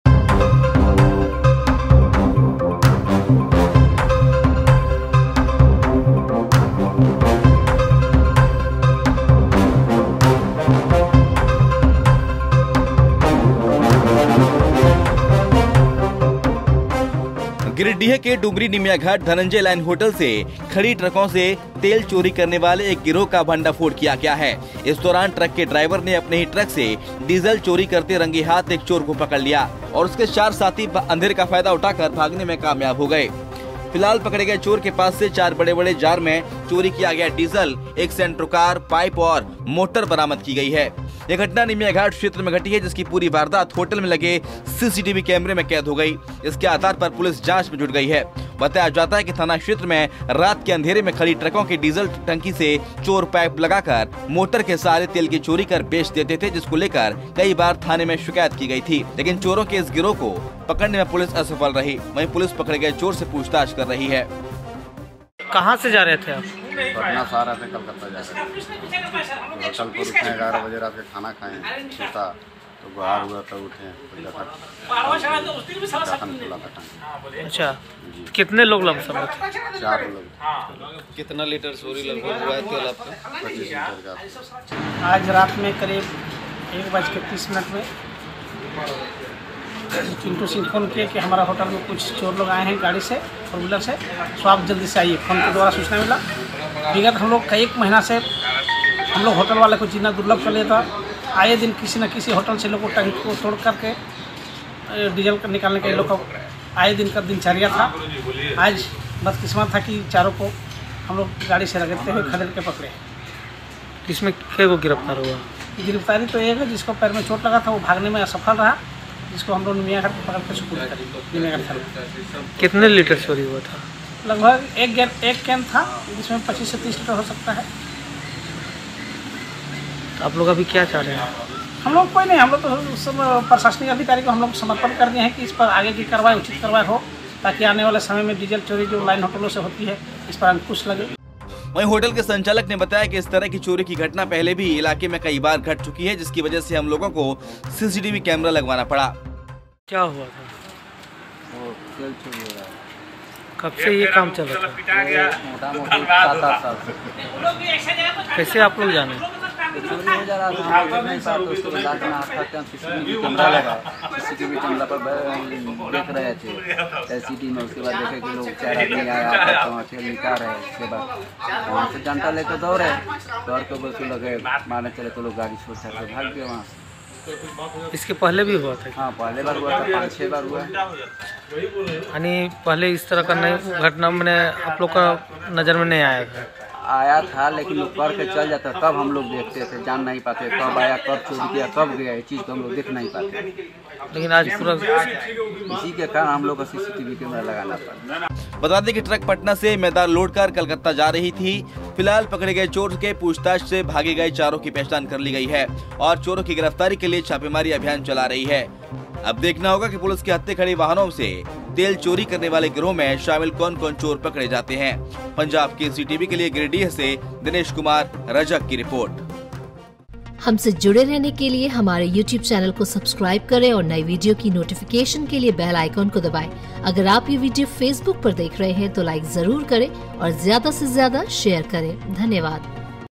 Jharkhand: खड़ी ट्रक की टंकी से Diesel की चोरी, ड्राइवर को लगी भनक..फिर दे दना दन
Jharkhand News: गिरिडीह ( Giridih ) के डुमरी निमियाघाट धनंजय लाइन होटल से खड़ी ट्रकों से तेल चोरी करने वाले एक गिरोह का भंडाफोड़ किया गया है….पकड़े गए चोर के पास से चार बड़े-बड़े जार में चोरी किया गया डीजल (Diesel ), एक सेंट्रो कार, पाइप और मोटर बरामद किया गया है.